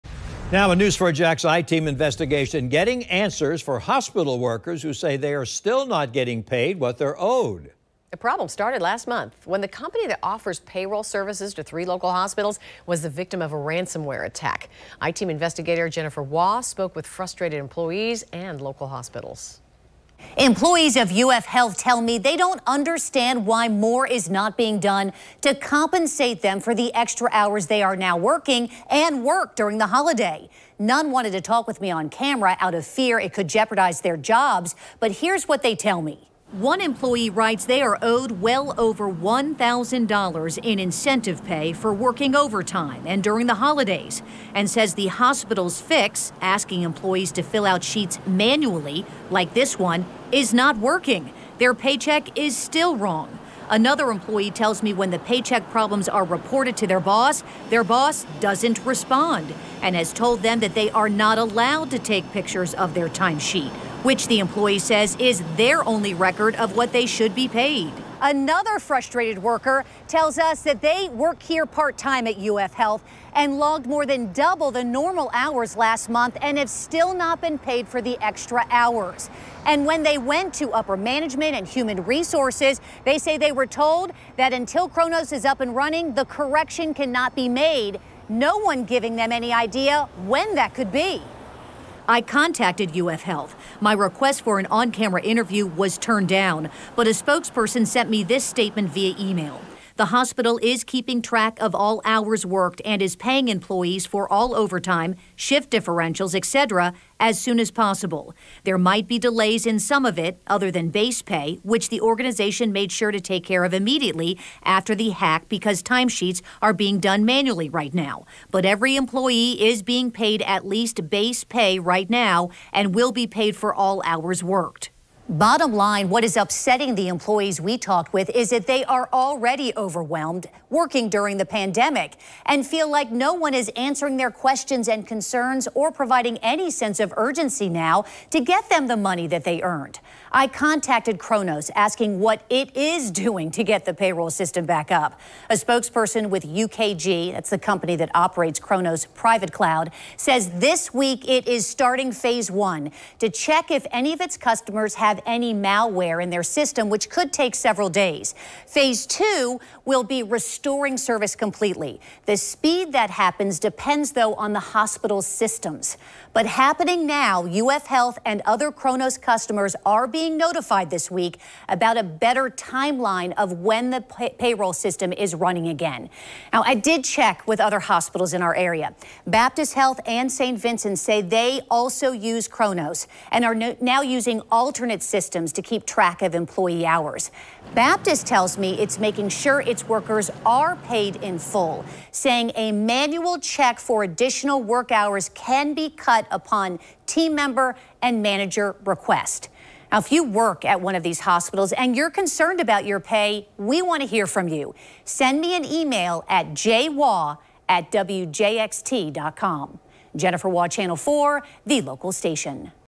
Local health care workers fed up with payroll delays triggered by ransomware attack
The I-TEAM has received calls and emails from health care workers who said they are frustrated that they are getting no answers from Human Resources and their bosses about when they will be paid in full for their work during the holidays.